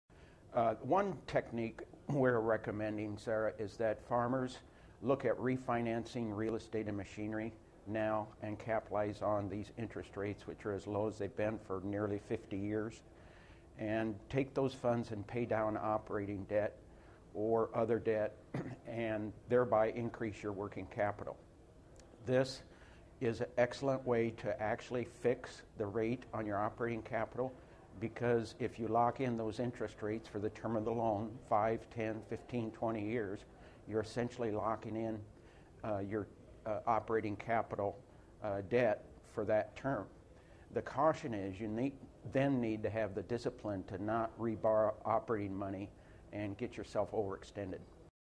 Take Advantage of Rock-Bottom Interest Rates
Interest rates are the lowest they been for 50 years, says Moe Russell, Farm Journal columnist and president of Russell Consulting.
He says farmers should look at refinancing real estate and machinery, and then use those funds to pay down operating debt.